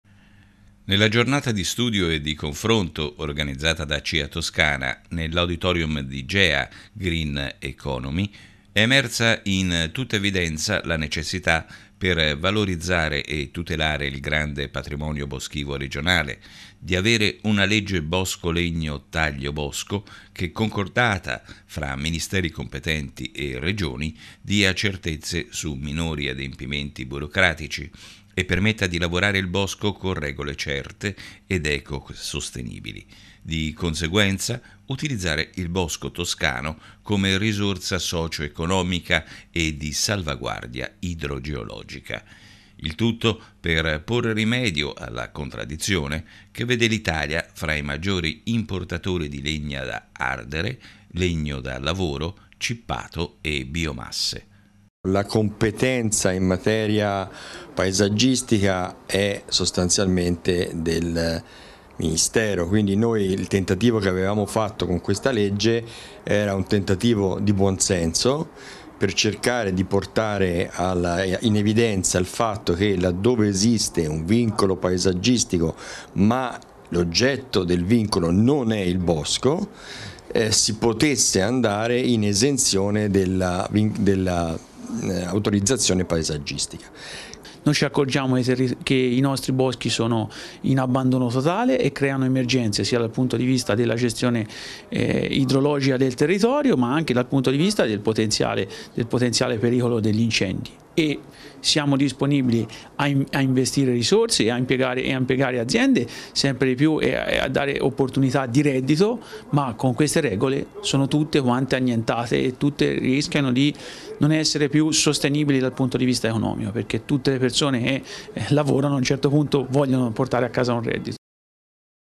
VALORIZZARE E TUTELARE IL PATRIMONIO BOSCHIVO TOSCANO
Valorizzare e tutelare il patrimonio boschivo. Se ne è parlato in un convegno organizzato da CIA Toscana